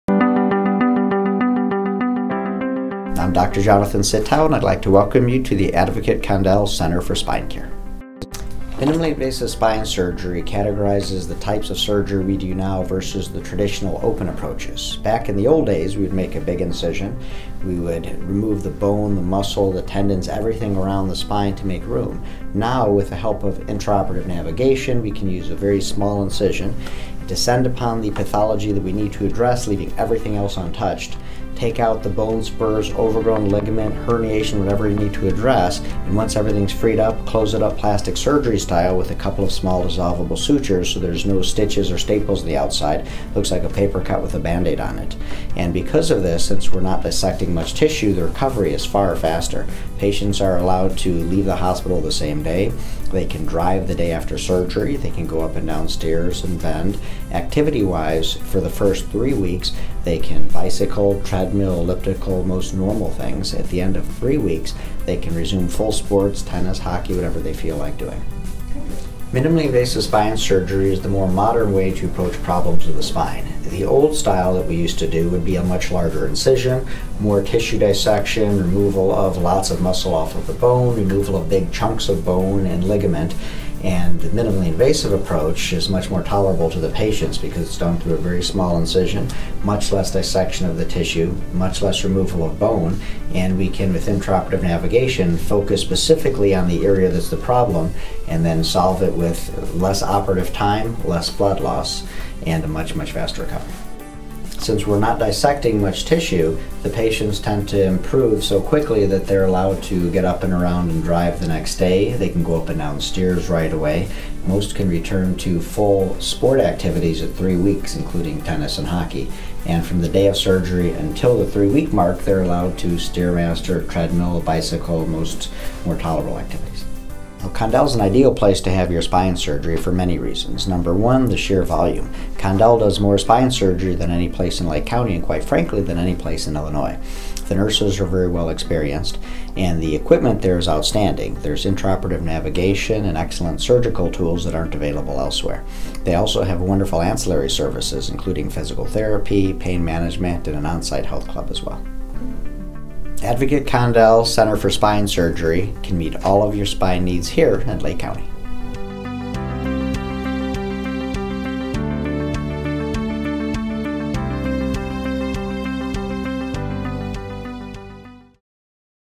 Minimally Invasive Spine Surgery
Dr. Jonathan Citow,  Chief of Neurosurgery at Condell Medical Center talks about how spinal surgery has evolved and why Advocate Condell Medical Center is the top hospital for this service.